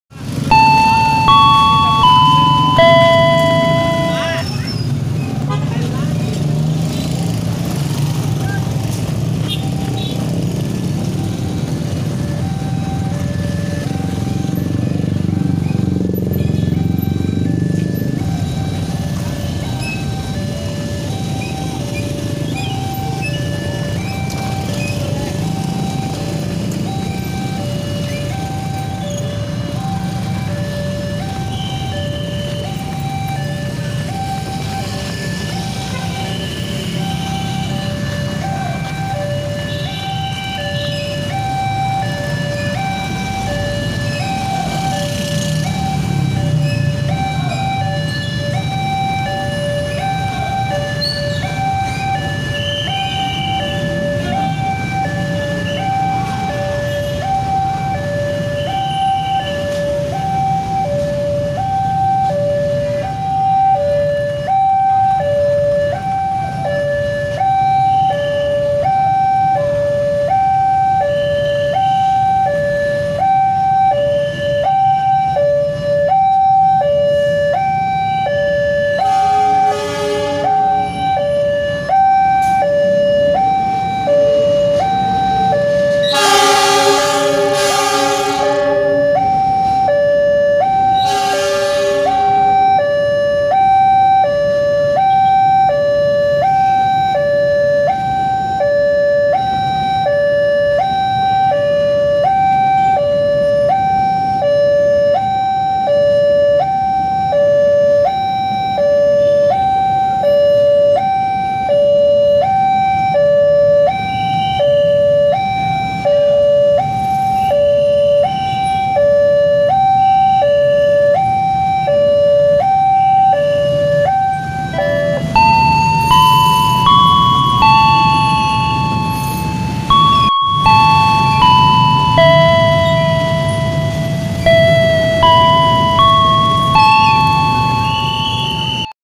Kereta Api Argo Parahyangan Melintas JPL 194 Di Bawah FlyOver Pantura Cikampek
Perlintasan KA JPL 194 Jl. Jenderal Ahmad Yani - Sudirman Bawah FlyOver Cikampek & Jalur Arah DaOp 2 Purwakarta - Padalarang - Bandung, Hari Sabtu 16 Juli 2022